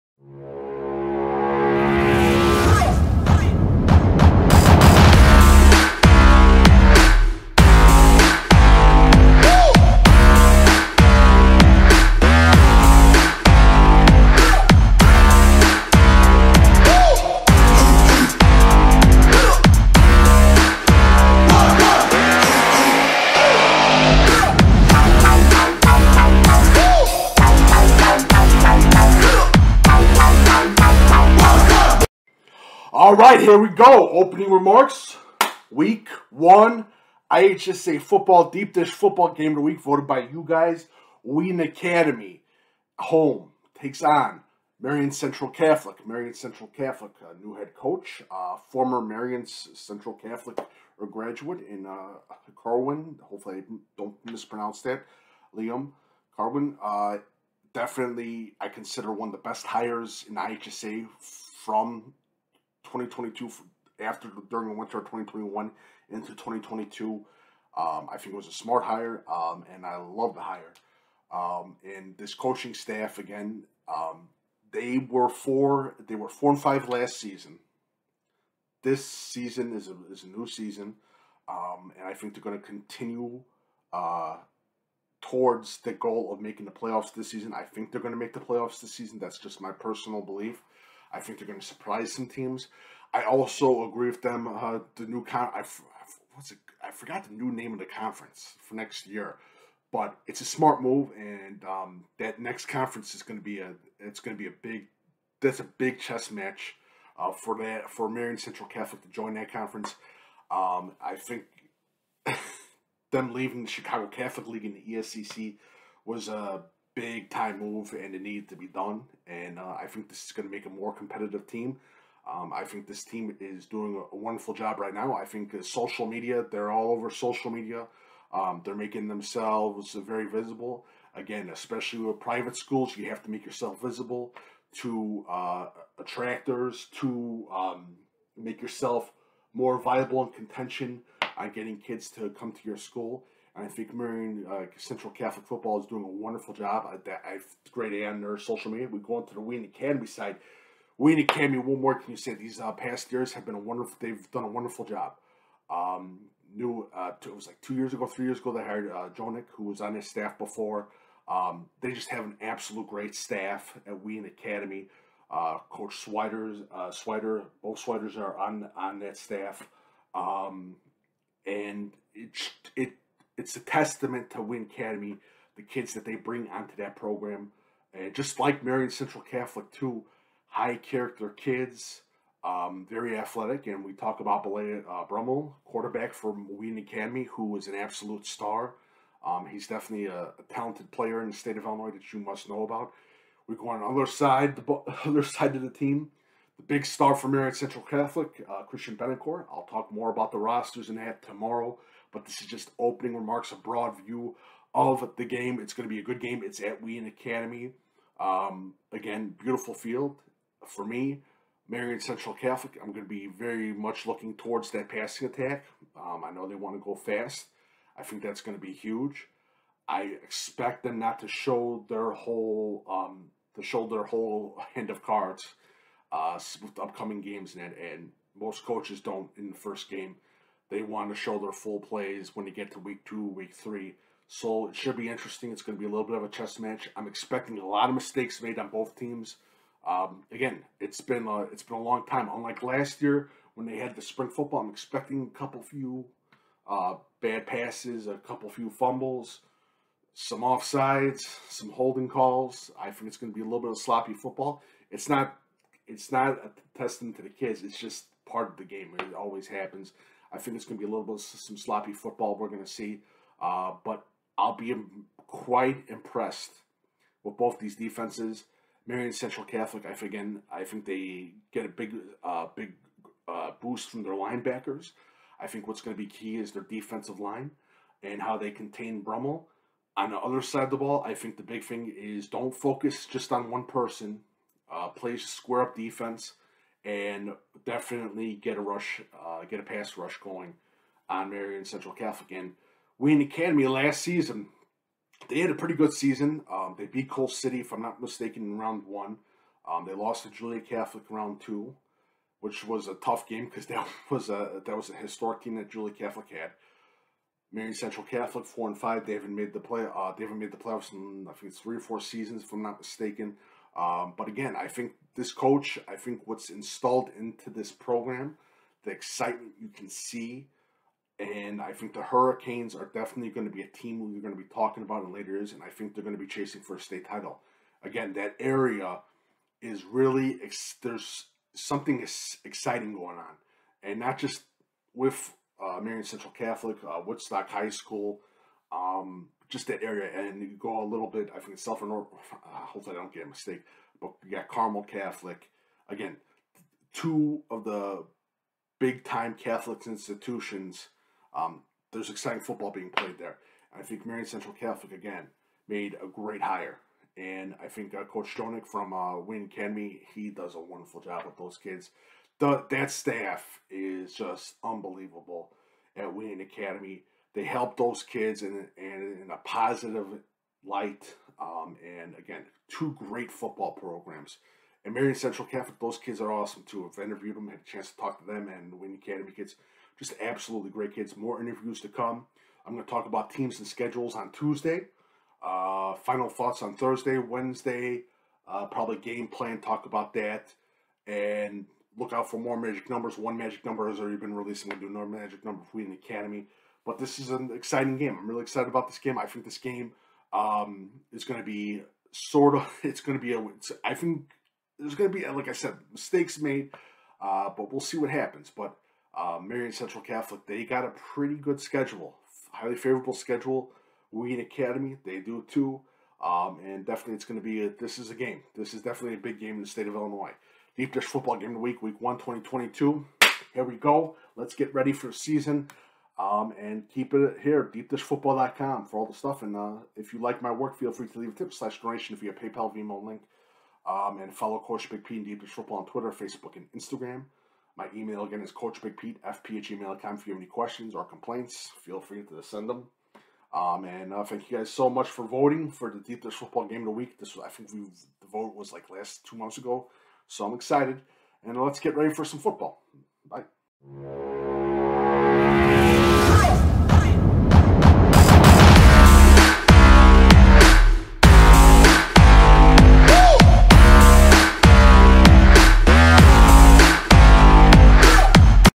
Opening Remarks for Deep Dish Football GOTW Week 1 Marian Central Catholic vs Wheaton Academy 2022
Time to kick off IHSA 2022 Fall Season Coverage
Deep Dish Football Game of the Week 2022
Marian Central Catholic vs Wheaton Academy
Opening Thoughts and Remarks on the IHSA Deep Dish Football Game of the Week as voted by you the people. Broad Overview
Check Out Facebook, Twitter, Instagram, YouTube as I dissect the Game, Coaches, and Players.

Follow @Coachbigpete and @deepdishfb on Twitter and Facebook